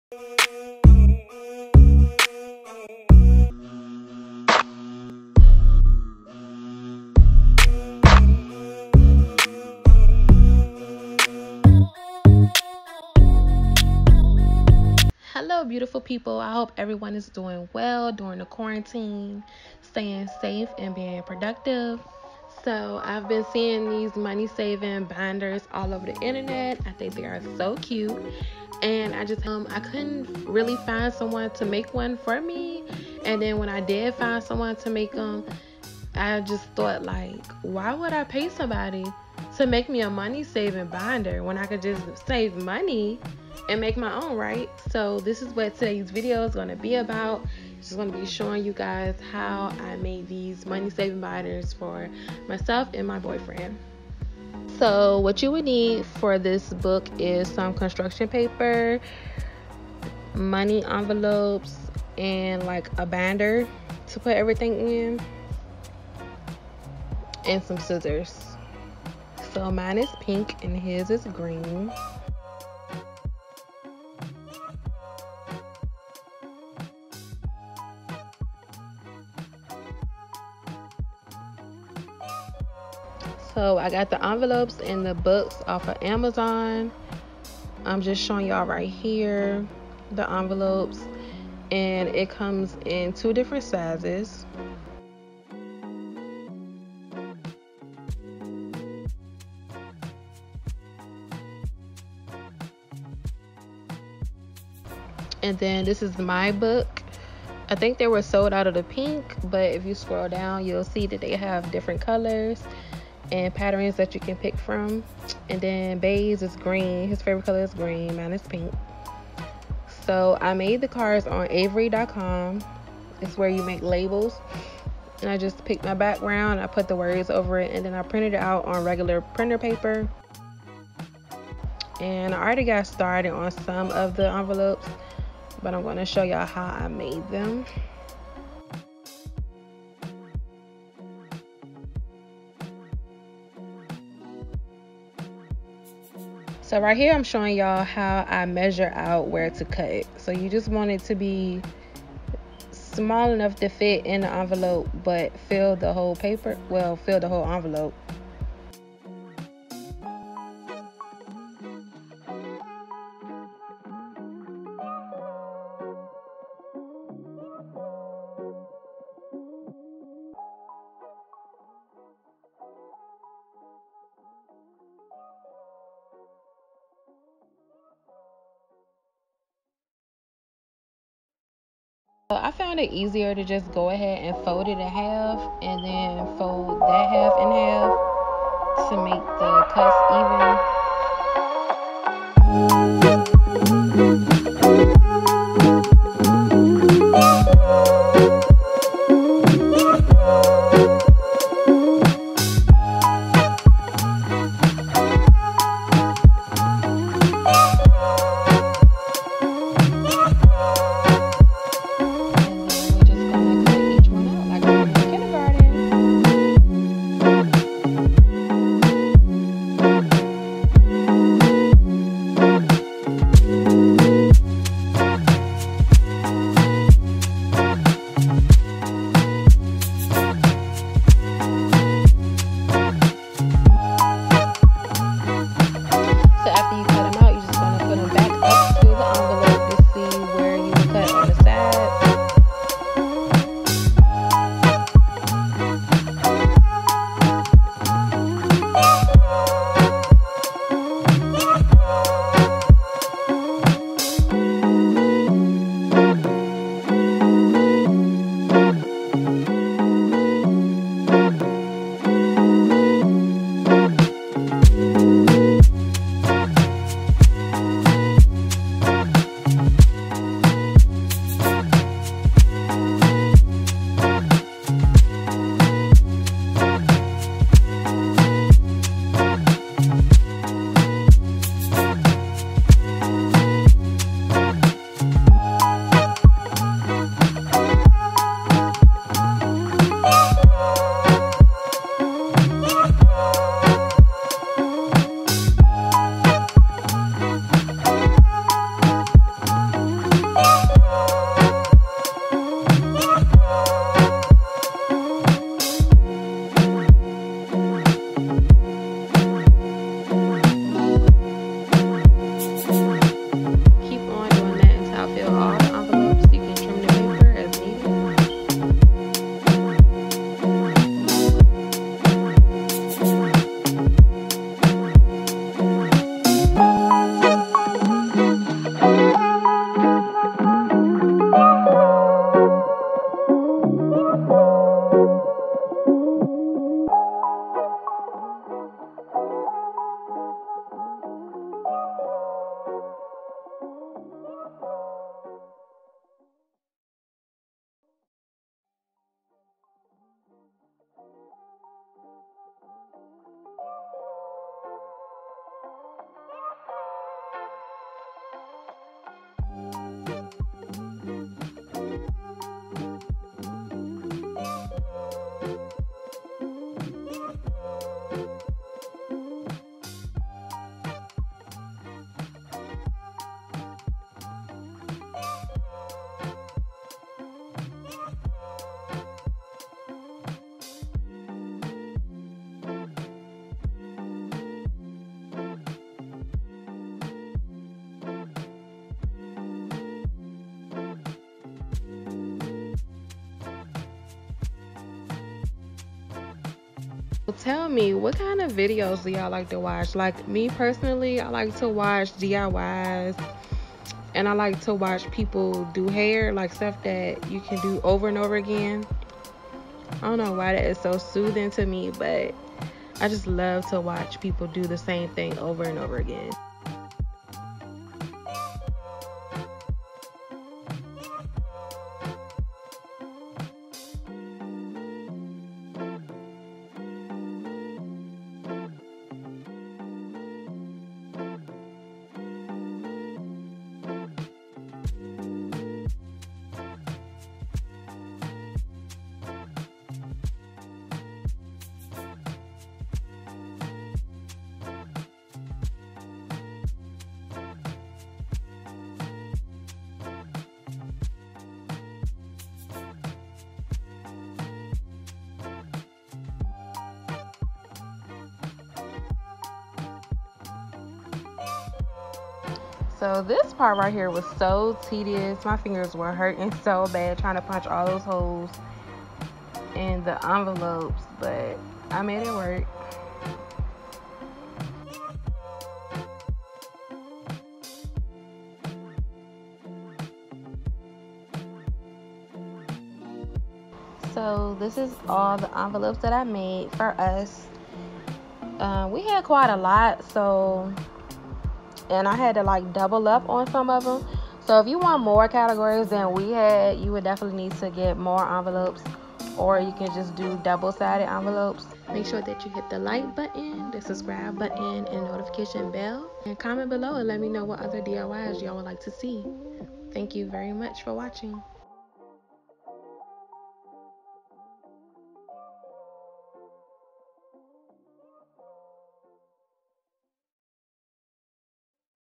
DIY Money Saving Binders | His & Hers
Welcome back beautiful people! This is a DIY video for these super cute money saving binders with envelopes. We are pretty much getting bored with the quarantine so it is time to get creative and crafty. Why spend money ordering a book when you can make one yourself? These are great for new business owners, new home owners, or for someone who just need more organization when it comes to budgeting!

Items used
Labels: Avery.com